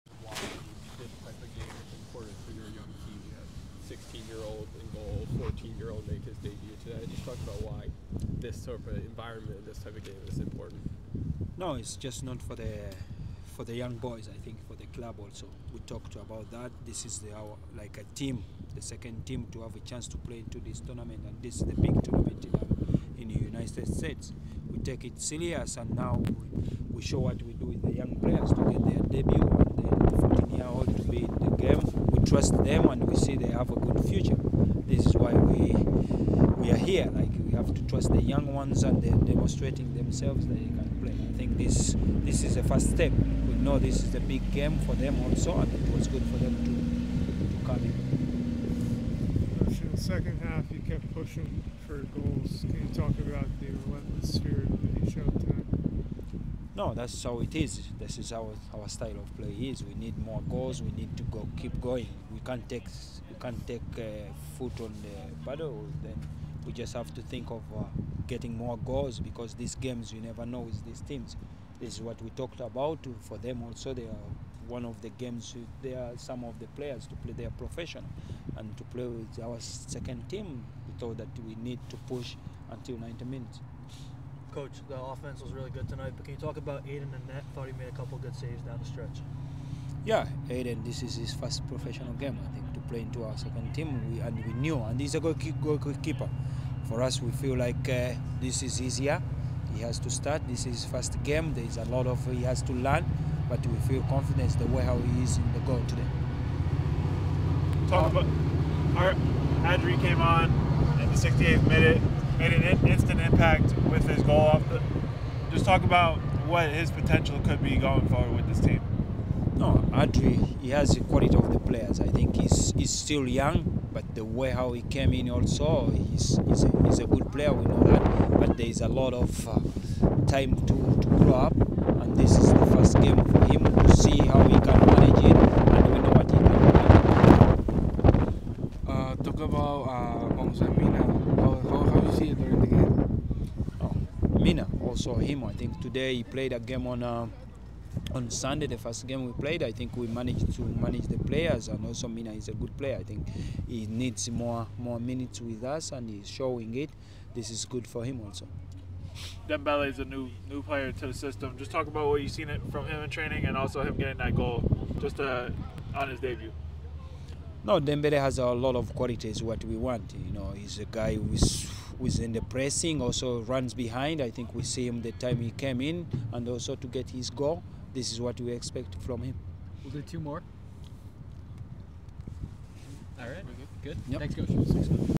Red Bull II Ibrahim Sekagya Full Post Game Media Availability vs Hudson Valley Hammers (US Open Cup)
Manager Ibrahim Sekagya talks about his team's 5-1 first-round victory over the Hudson Valley Hammers in the US Open Cup. (3/20/24)

Check out  @areasportsnetwork9854  for country-wide MLS and soccer content.